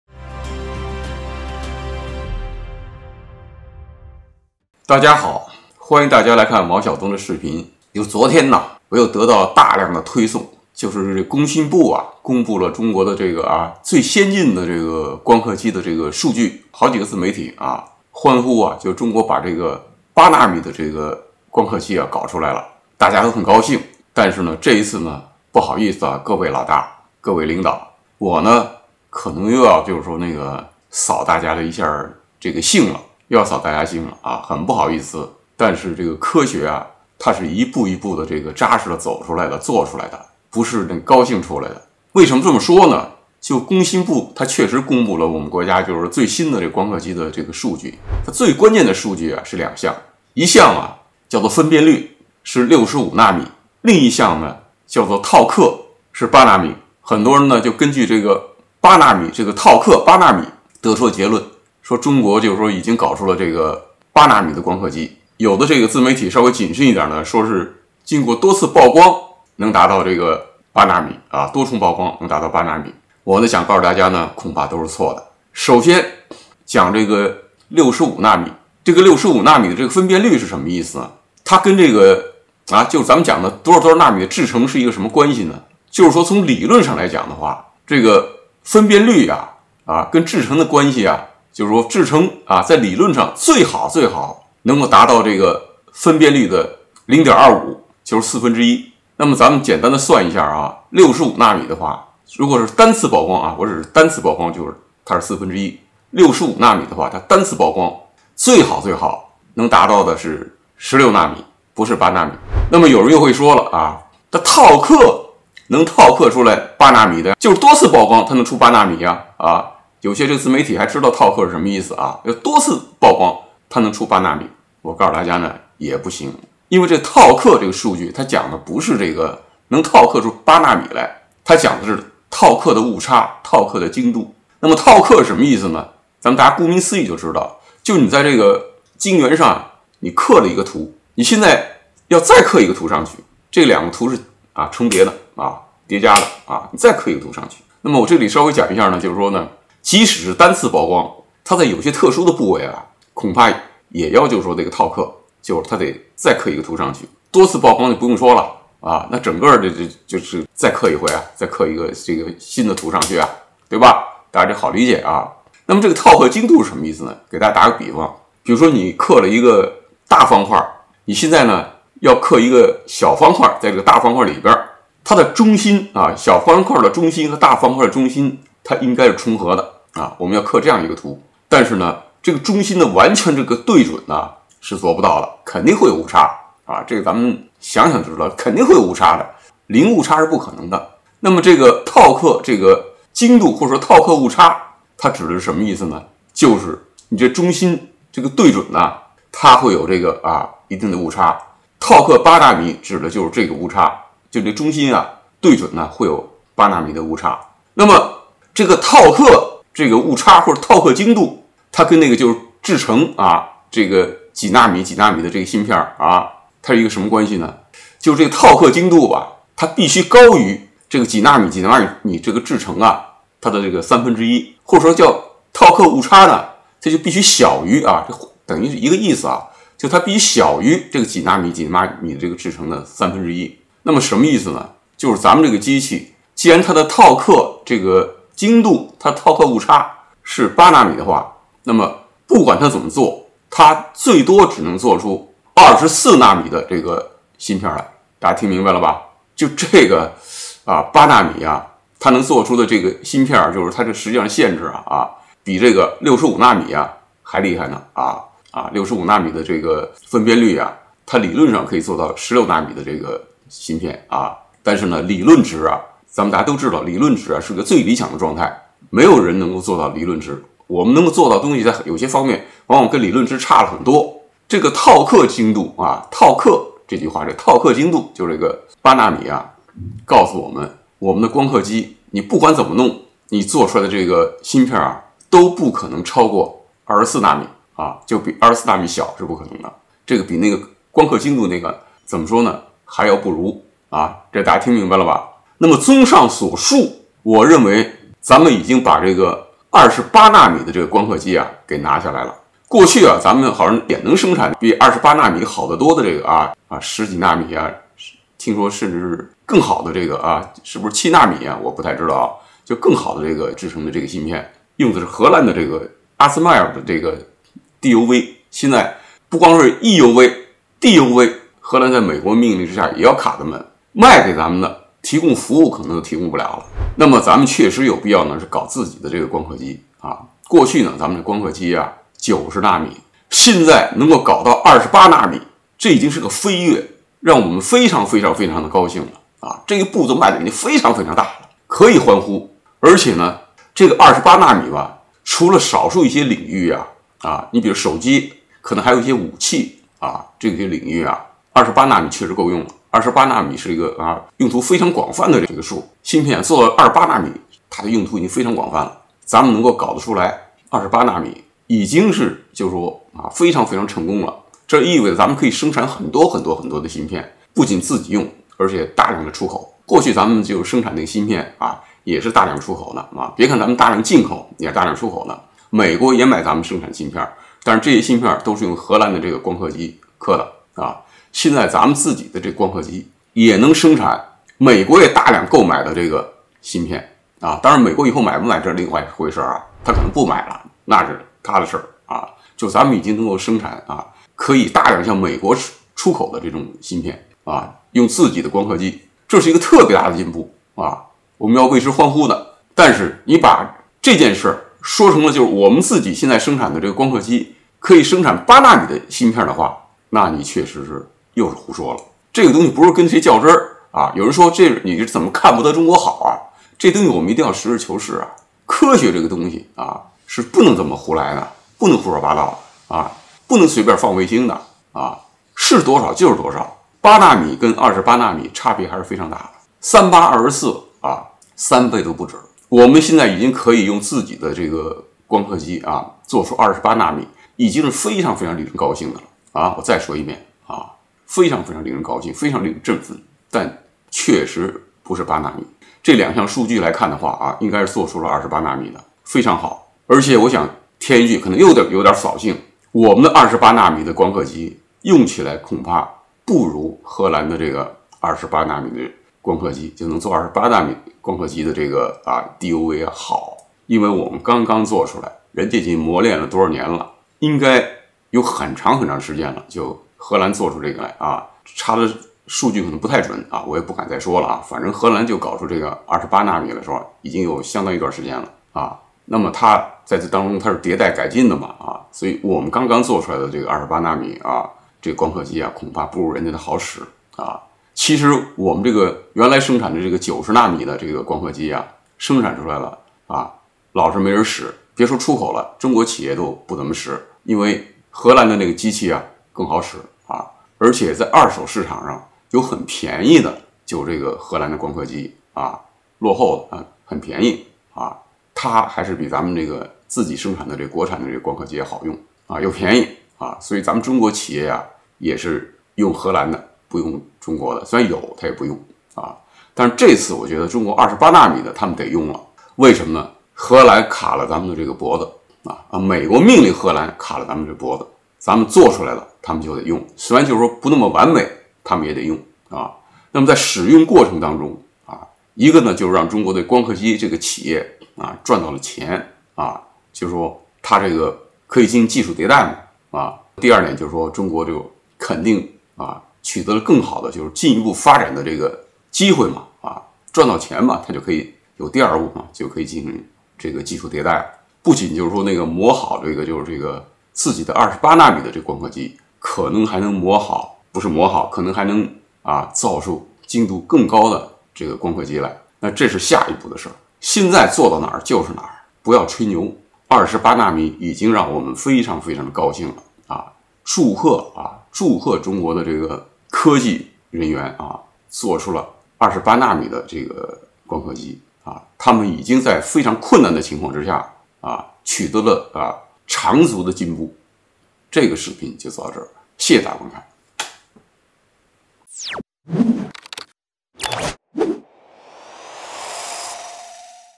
國產光刻機突破了8奈米？遺憾，進步很多，但不是8奈米
大家好，歡迎大家來看王小東的視頻
昨天我又得到大量的推送，就是這工信部公佈了中國的這個，最先進的這個光刻機的這個數據